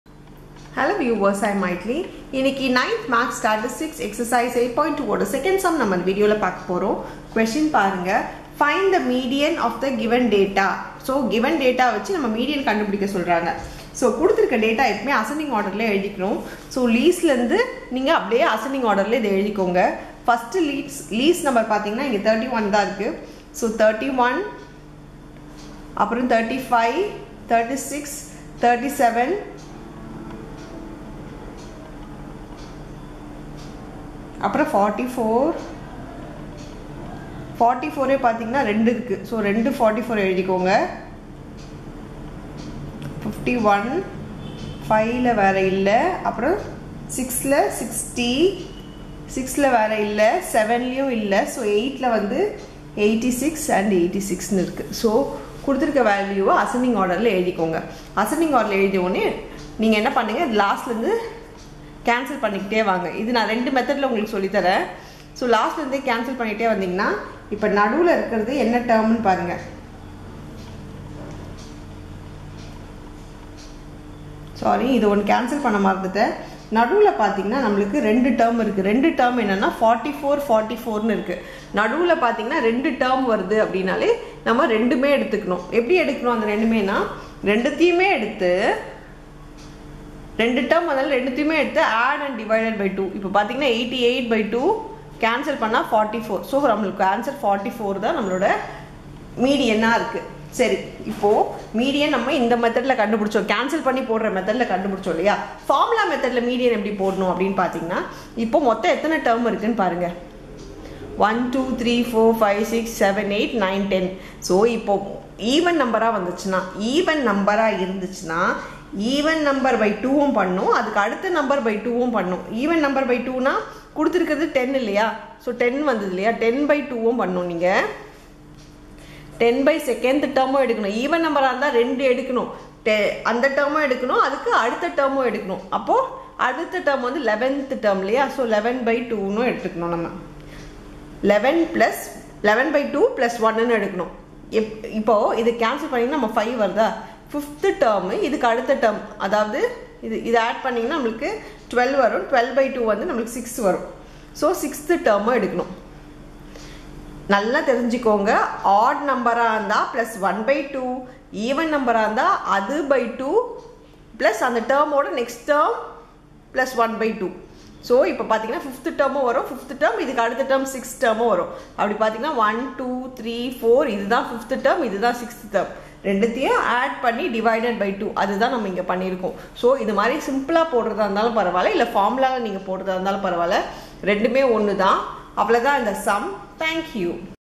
TN Samacheer 9 Maths New Syllabus Statistics Ex 8.2 Sum 2 .
Tamil Nadu State Board Samacheer 9 Maths New Syllabus Complete Solution for Ex 8.2 Sum 2 in Statistics.